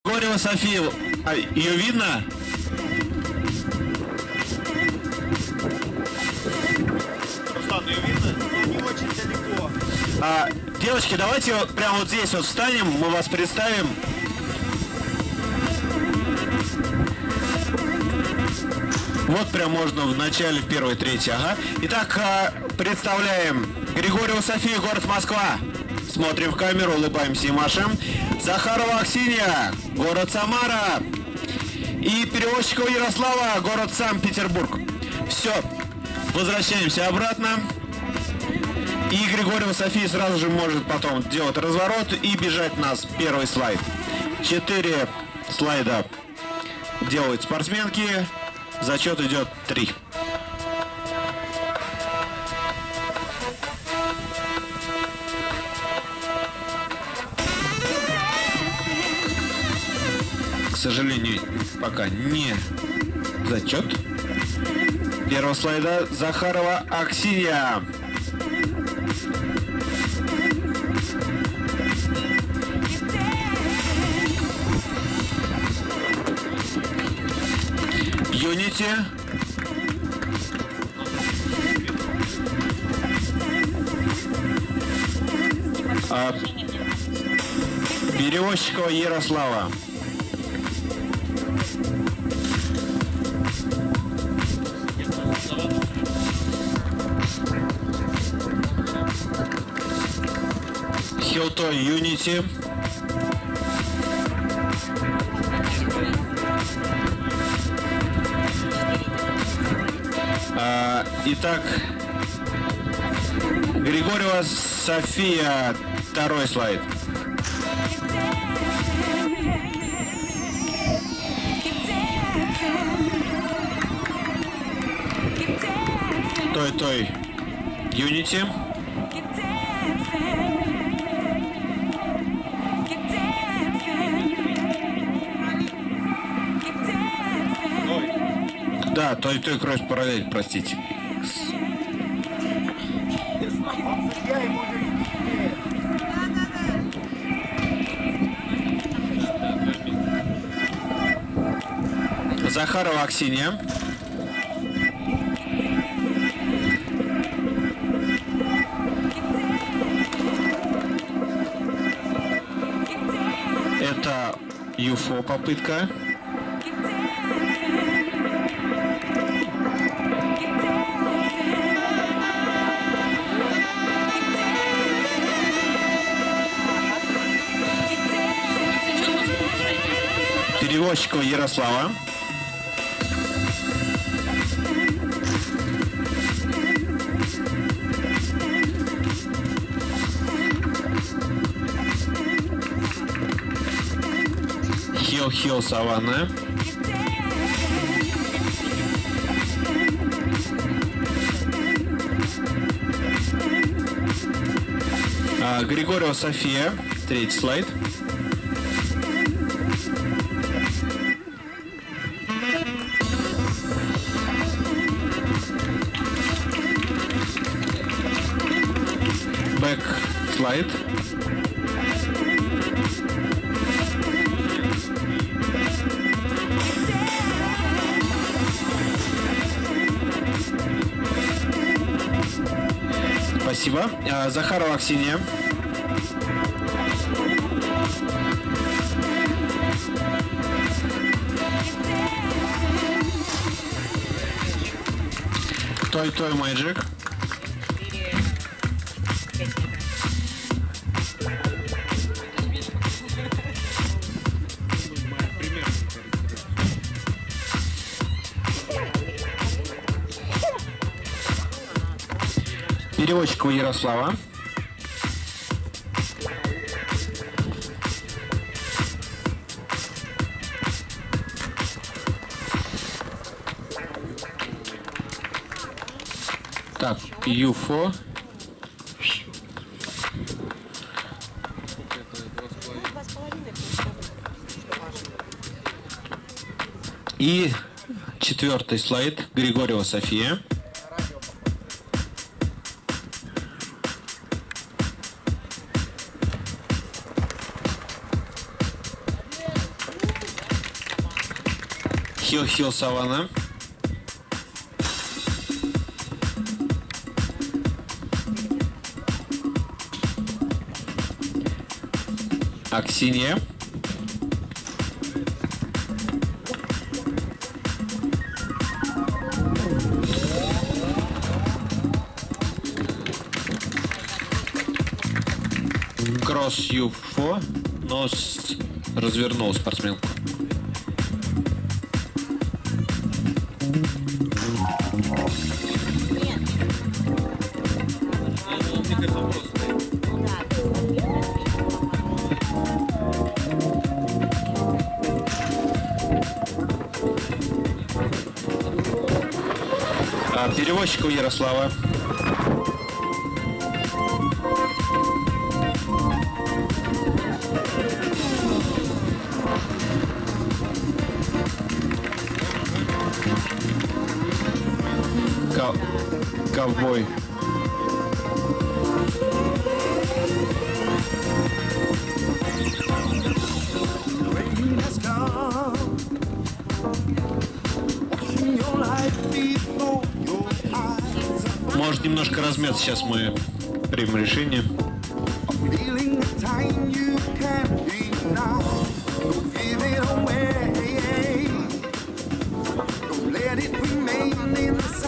Саратов Стайл Контест XVII, 2023. Слайды. Женщины. 1/2 Финала. Часть 1.
Саратов Стайл Контест XVII, Фристайл Слалом. Место проведения: г. Саратов. ФК Юбилейный. 17-18 июня 2023г